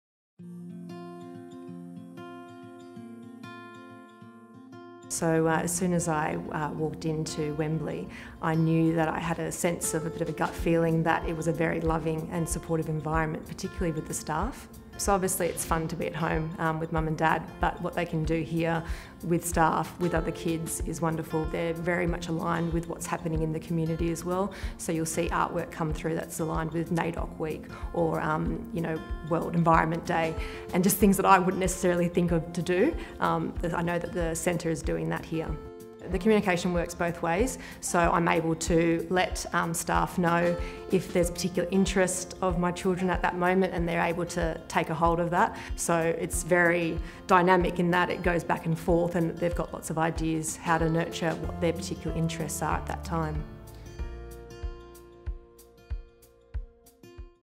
MercyCare Wembley Early Learning Centre Parent Testimonial
Our parents tell us their experiences at our Early Learning Centre.